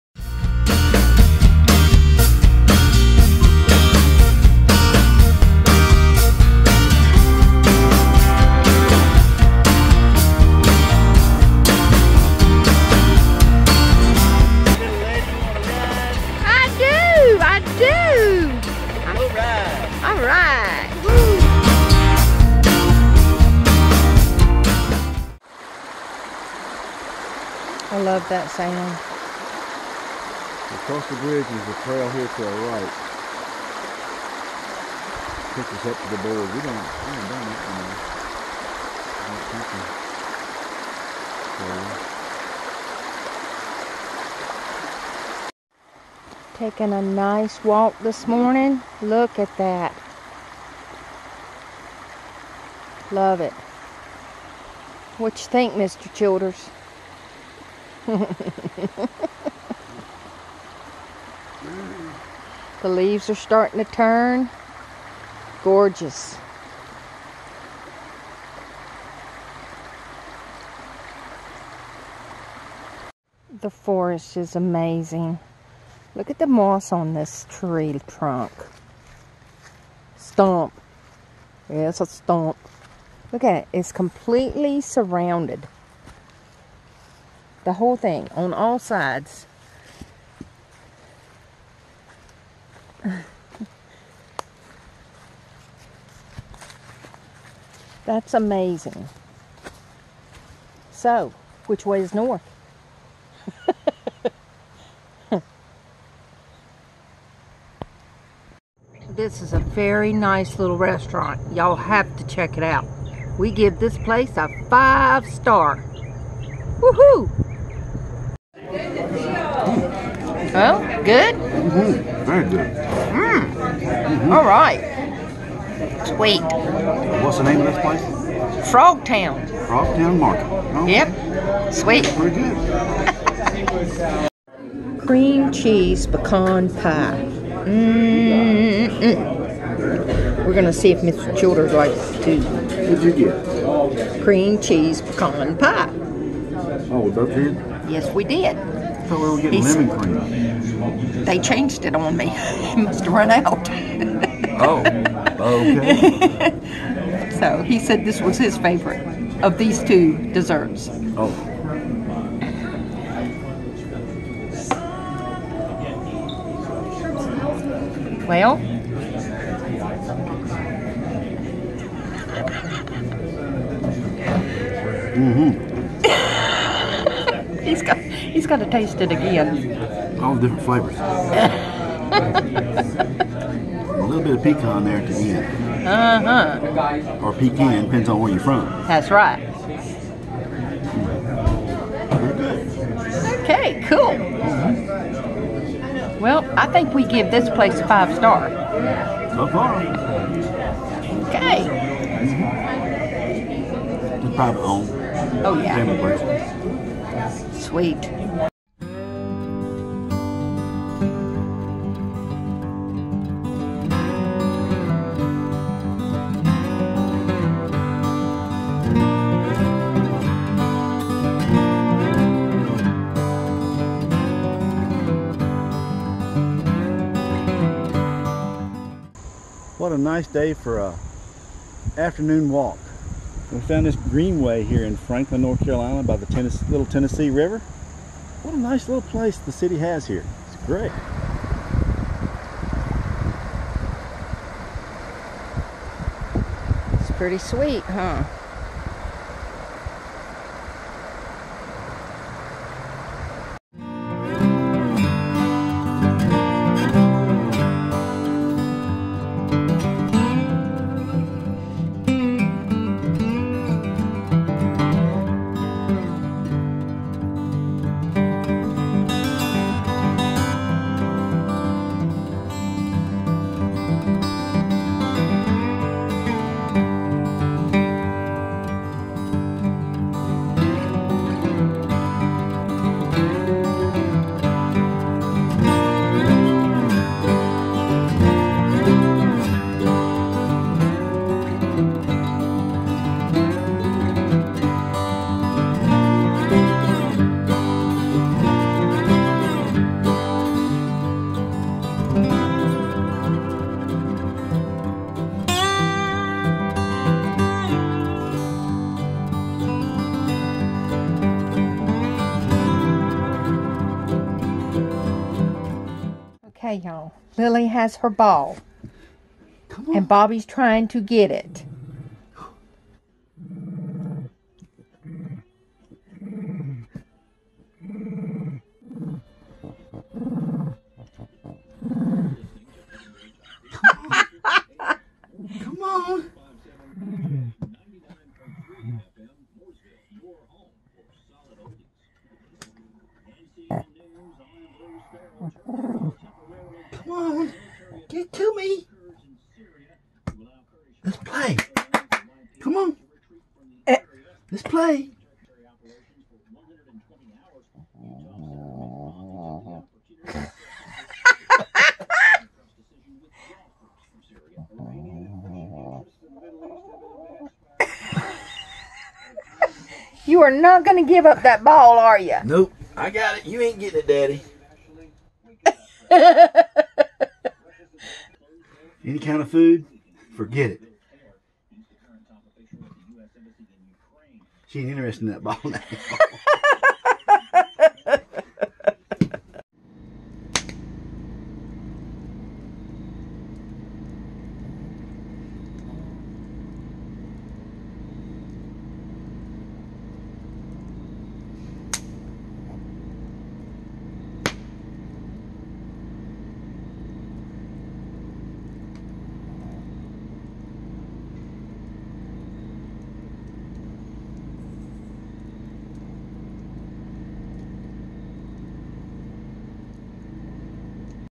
#446 Franklin, NC, Frogtown Market
RV life and adventures with The Nomadic Ramblers: Just enjoying Franklin NC. and playing with Lilly our little Shih Tzu.